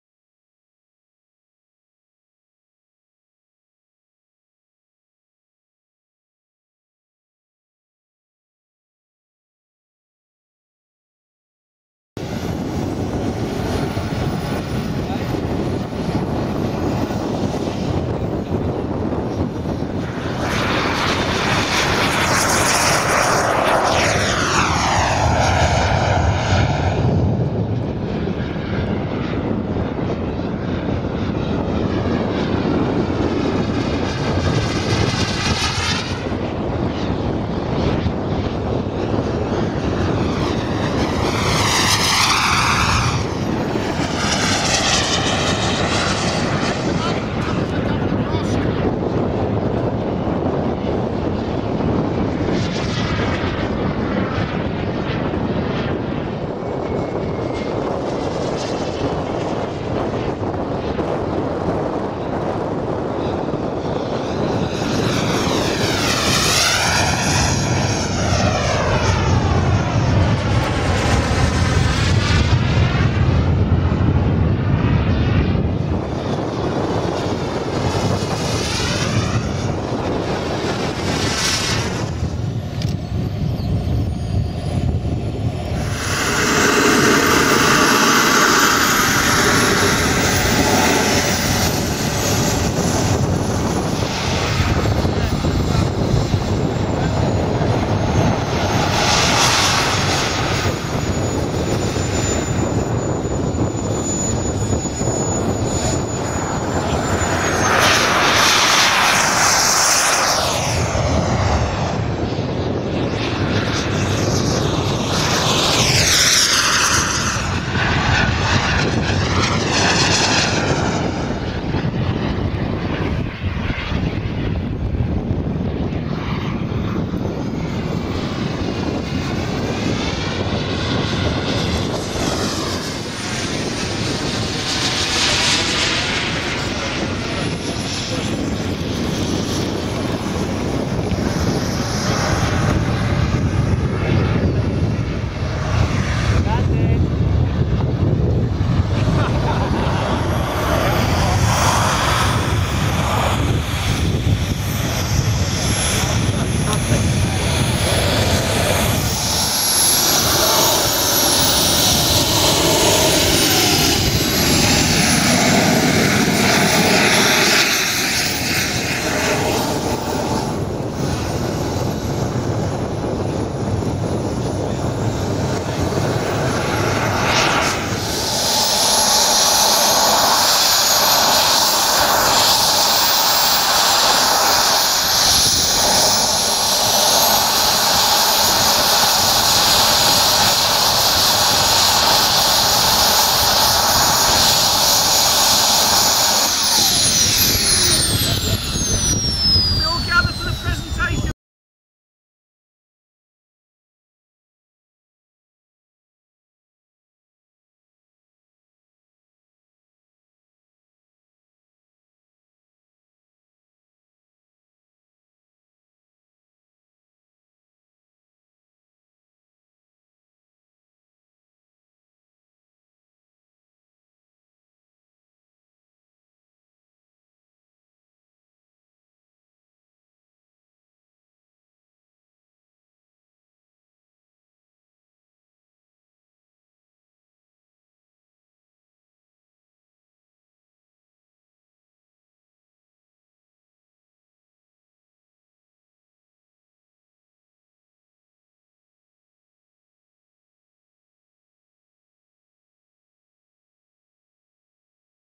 LARGE SCALE RC GENERAL DYNAMICS F-16 FIGHTING FALCON AT BARKSTON WARBIRDS MEET - 2014
FILMED AT AN RC FLY-IN AT RAF BARKSTON HEATH - GRANTHAM - LINCOLNSHIRE - NG32 2DQ  ON 9-3-2014, MANY THANKS TO RICHARD SCARBOROUGH FOR ALLOWING US TO FILM THIS WELL TURNED OUT EVENT,
LOW WINDS AND LOTS OF SUNSHINE MADE FOR A GREAT DAY OUT,